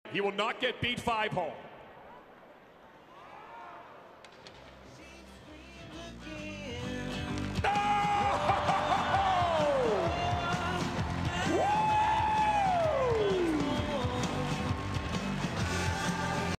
JJ Watt Skates with the Badgers
JJ Watt scores a goal at the Blake Geoffrion Classic